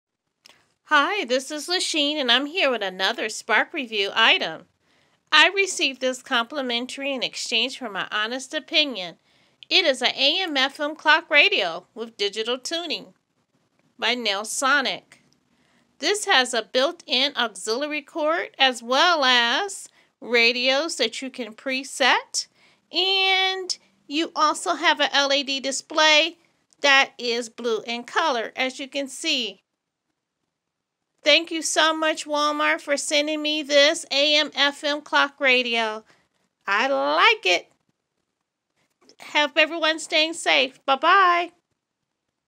Walmart review-am/fm clock radio
I received this complimentary in exchange for my honest opinion. it has so may features!